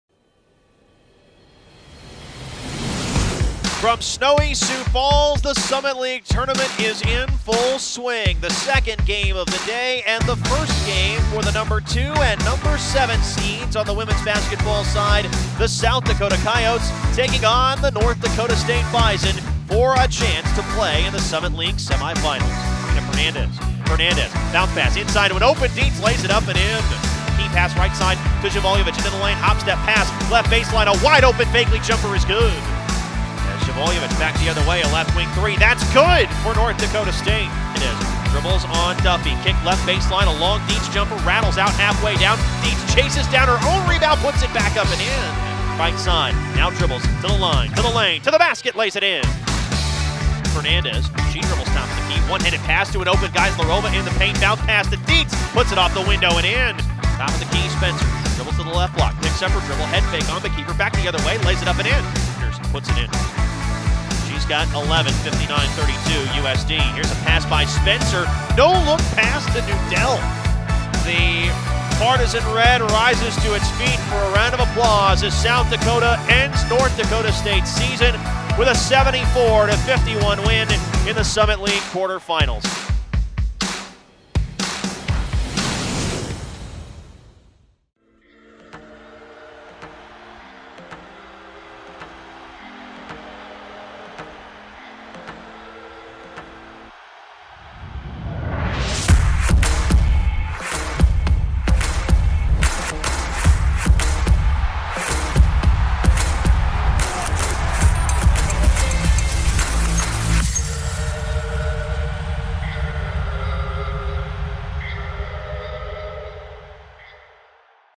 NDSU Women's Basketball Falls to USD in Quarterfinals of Summit League Tournament
Fighting for their season, the NDSU Bison Women's basketball team took on #2 seed USD and fell to the Coyotes, 74-51.